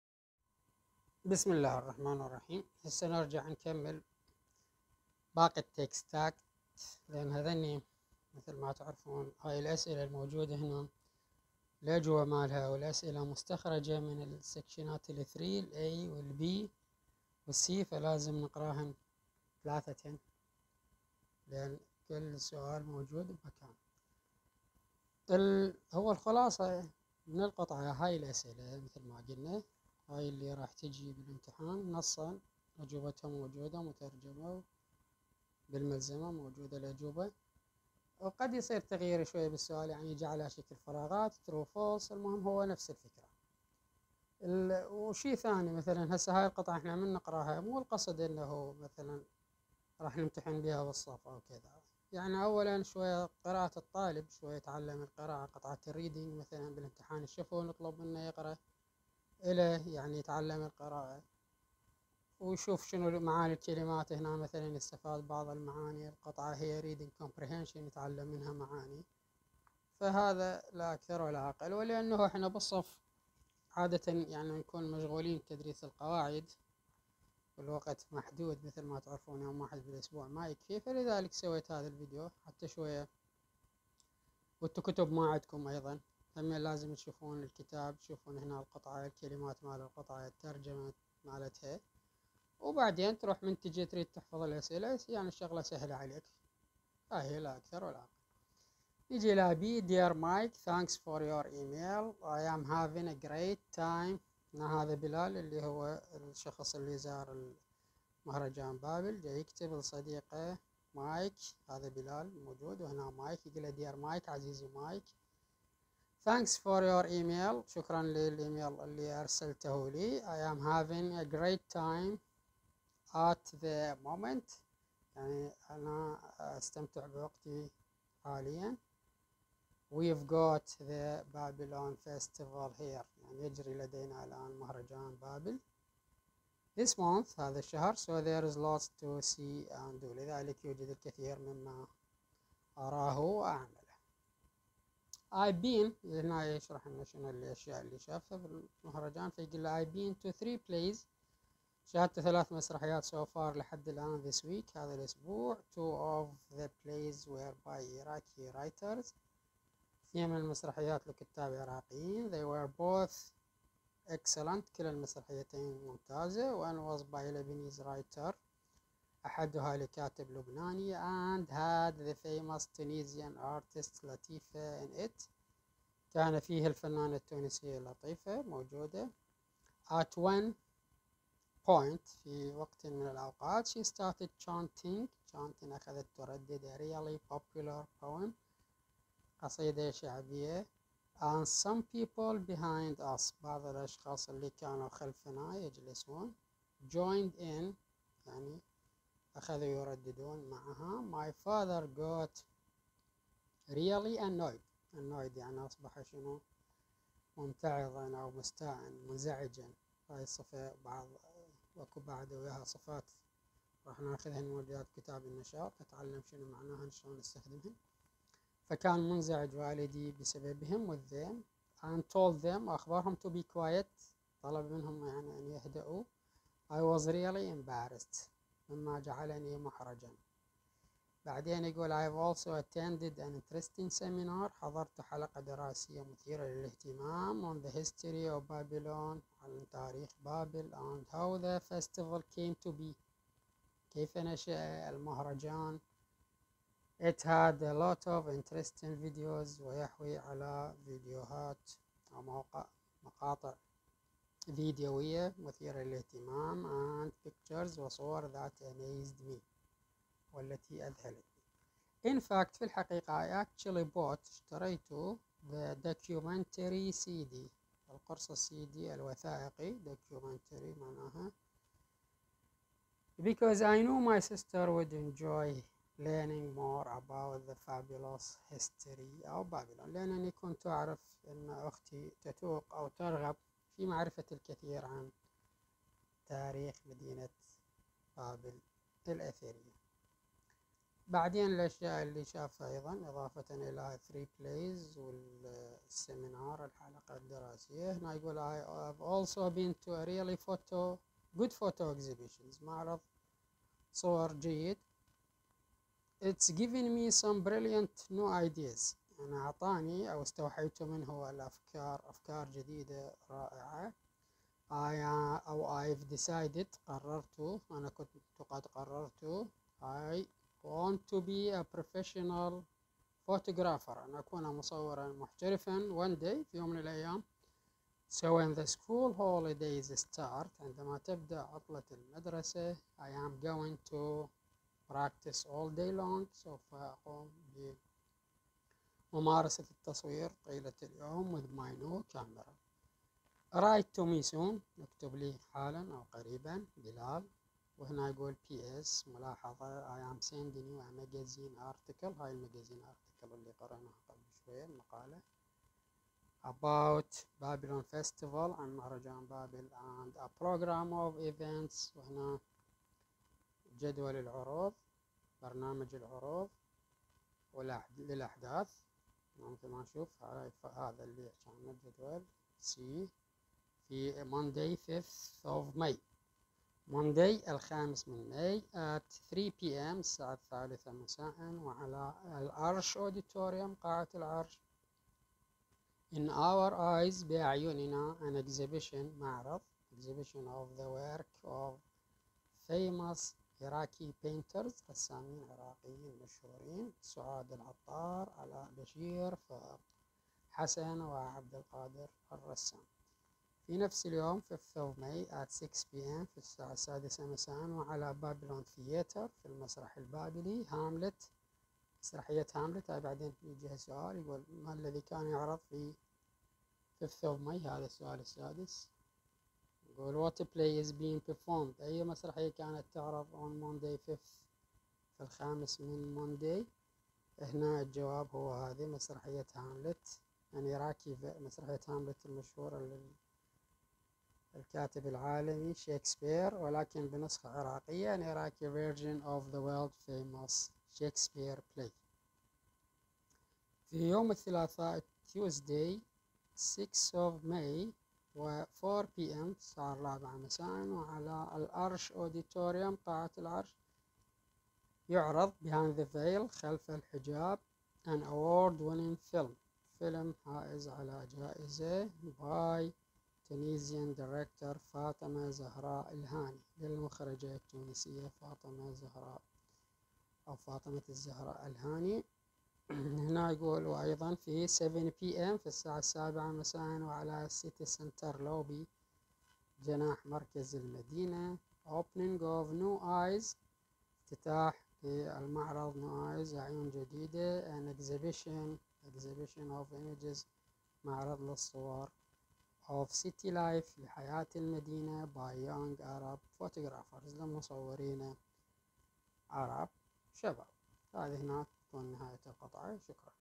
درس قطعة Babylon festival الخامس الاعدادي الجزء 2
قطعة مهرجان بابل اللغة الانكليزية للخامس الاحيائي والتطبيقي و الأدبي الوحدة الأولى Babylon festival مع الترجمة وحل اسئلة القطعة